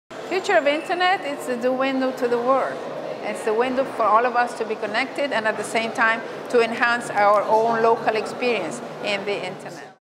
Global IGF 2015: Olga Cavalli on Internet Future in a Nutshell
Olga Cavalli, director of the South School on Internet Governance, shares her answer to the question, “Using 10 words or less, in just five seconds, briefly describe the future of the Internet.” Recorded at the Global Internet Governance Forum 2015 in João Pessoa, Brazil, by the Imagining the Internet Center, an initiative of Elon University, North Carolina, USA.